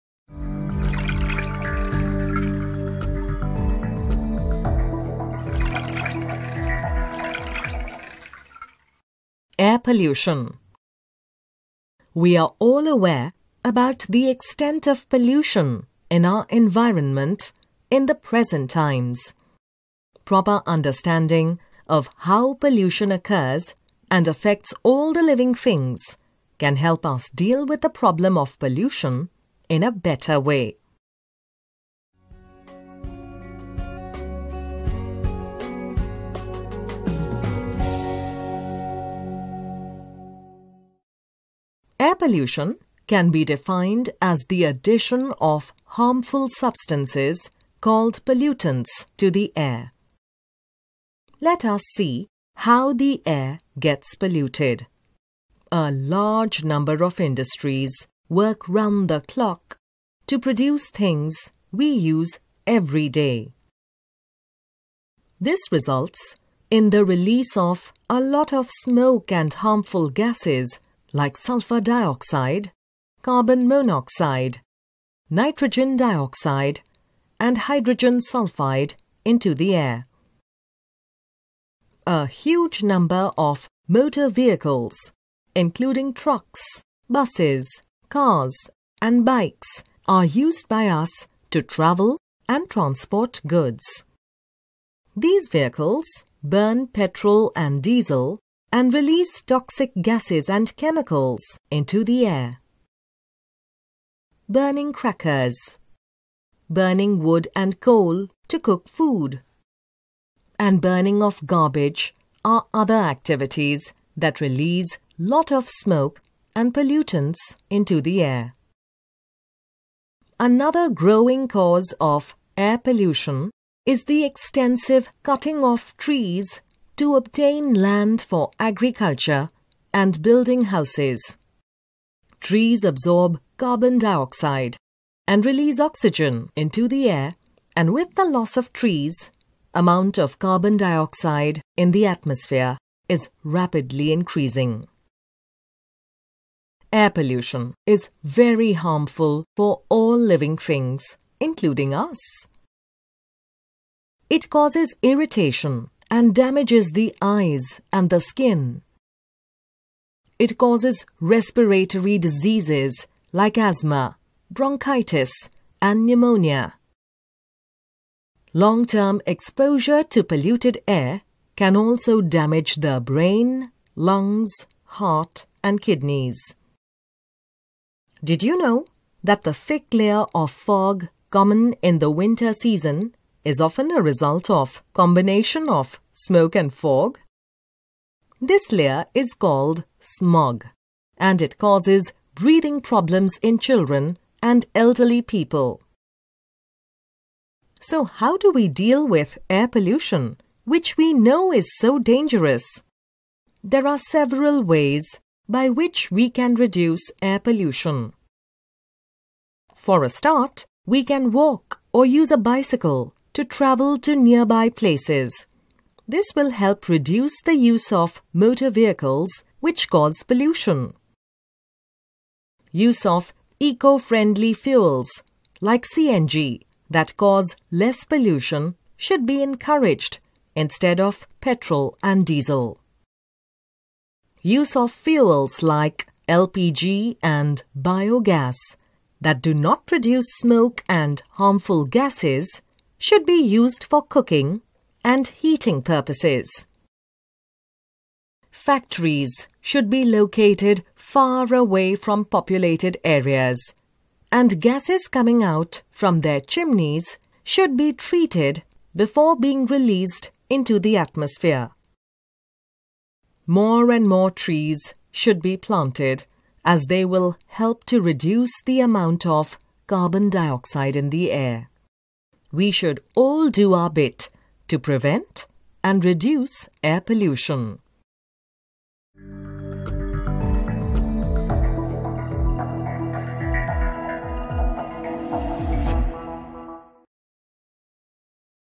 Air Pollution | Macmillan Education India
The air that we breathe in is important and needs to be kept clean. How air pollution occurs and how to curb it is something we all need to understand and follow. Check this informative animation on air pollution!